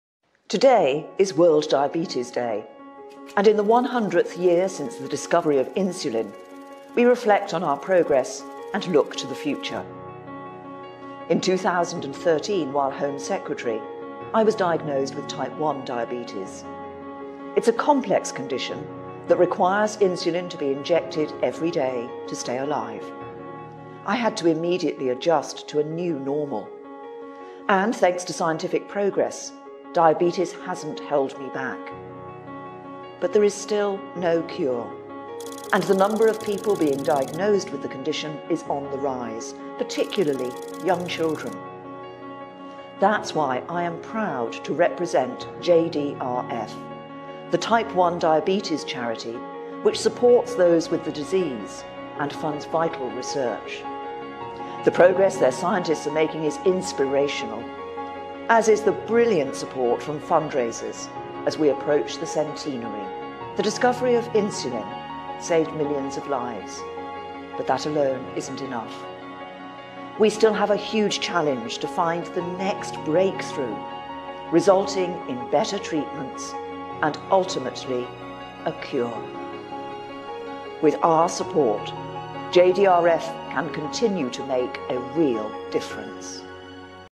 Theresa May - World Diabetes Day
Today is World Diabetes Day. Theresa May speaks about her experience with Type 1 Diabetes, and talks about